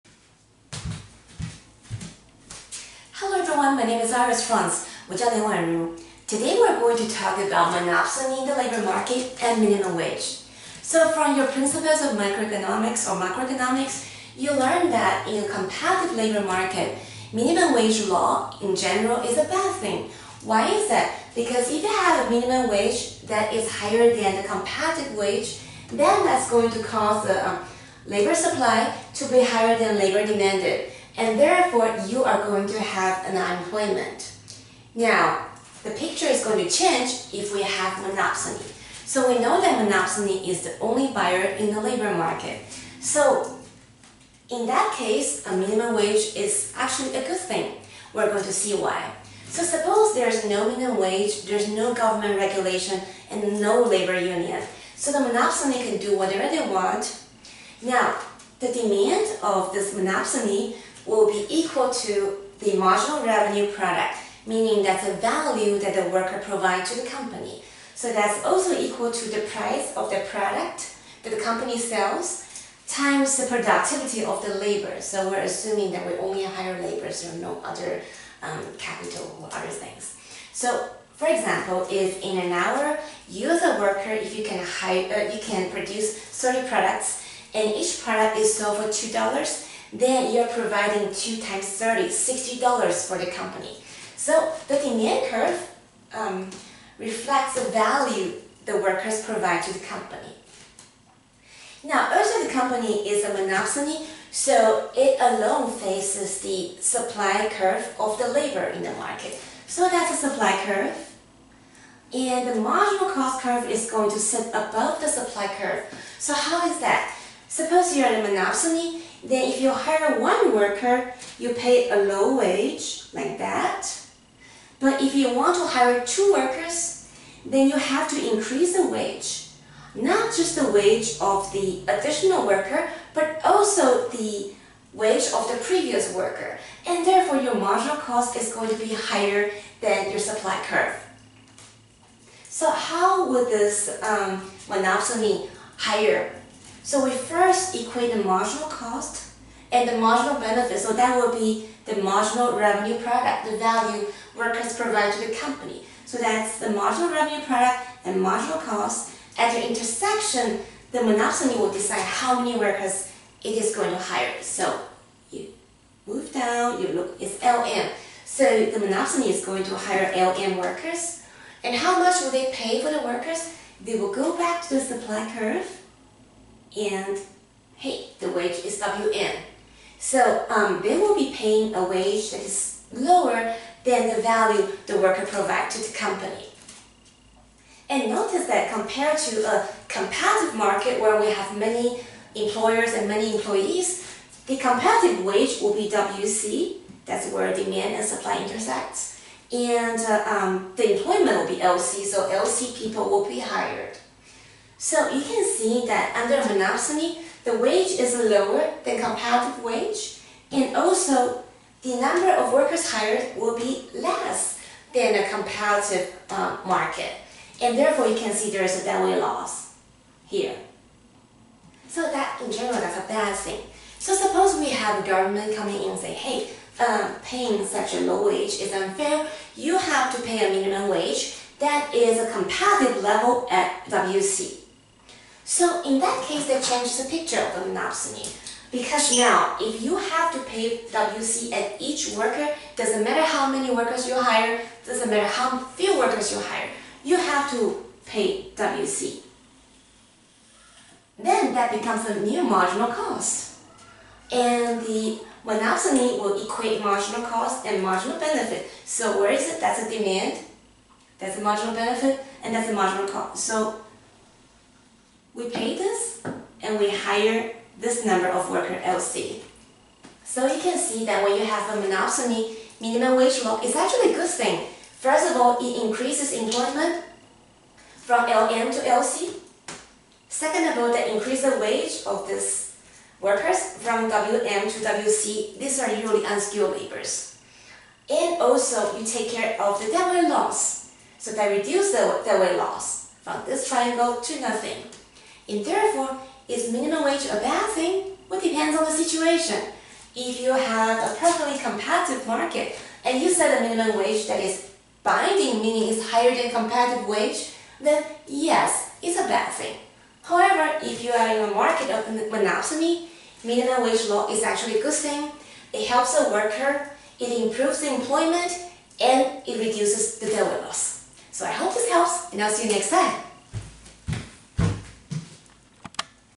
Monopsony and the Minimum Wage Law (Hal Varian Figure 27.3)
With a monopsony in the labor market, the minimum wage law increases employment, raises wage, and reduces deadweight loss.